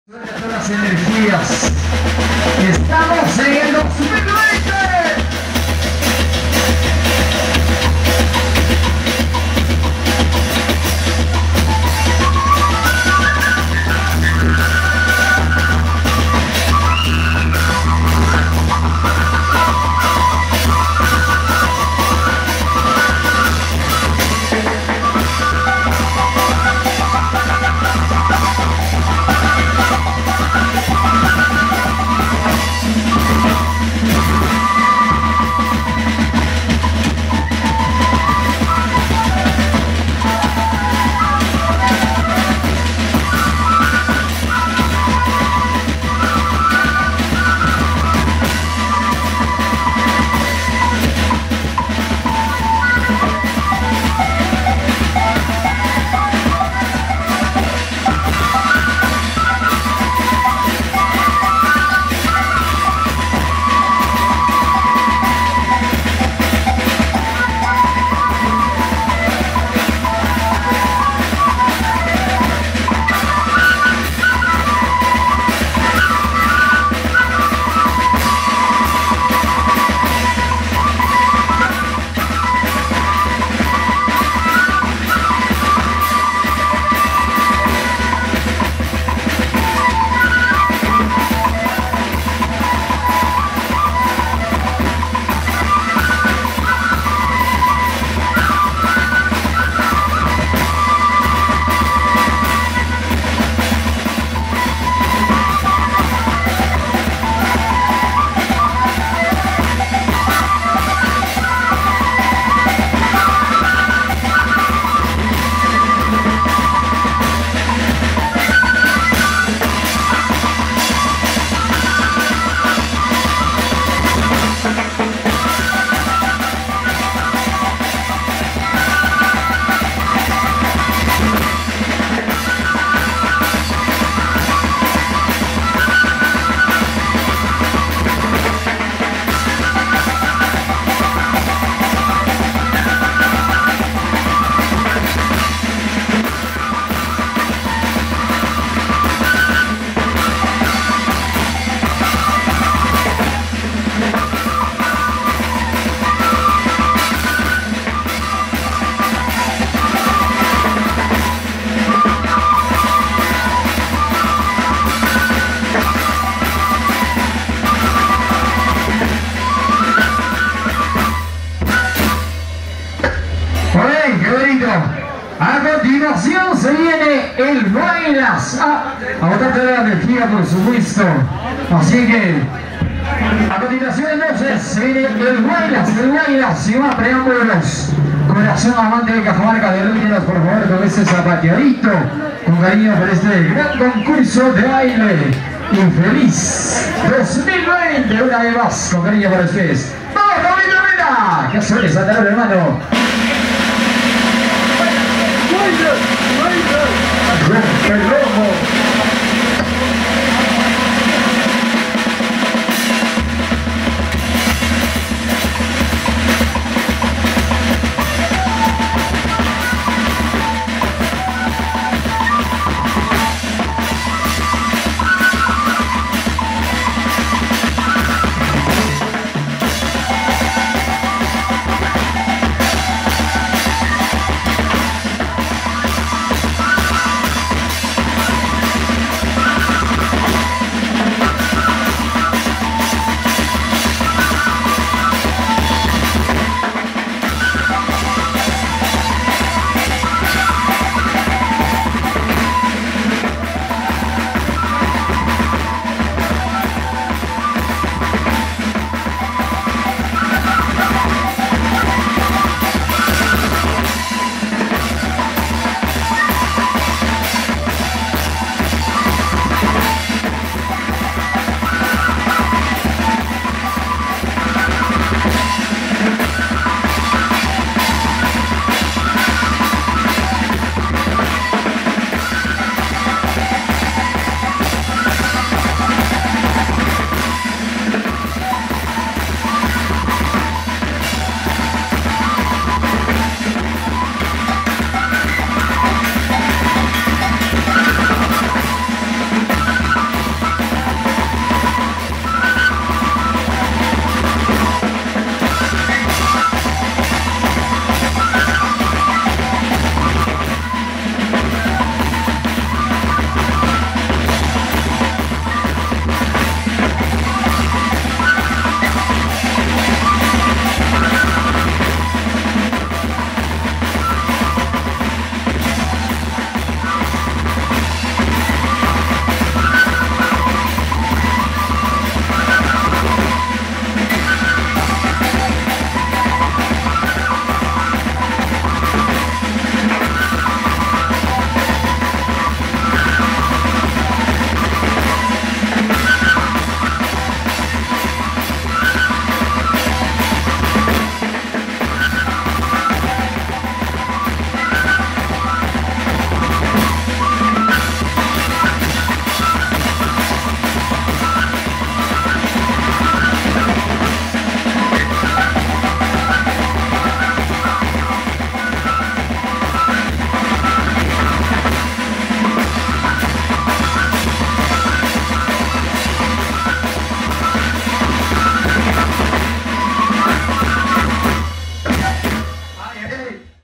CONCURSO DE BAILE CON BANDA TIPICA / BT Corazon Amante de Cajamarca - La Colpa Chanta Alta
Banda Típica: Corazón Amante de Cajamarca
TEMAS: Huayno y Huaylas
PRODUCCIÓN: Producciones "LOS ÁNGELES"
REALIZACIÓN: "Johann_Vargas´17ᴴᴰ"
LUGAR: Primer Aniversario de Creación Política del Caserío La Colpa - Pueblo de Chanta Alta 2020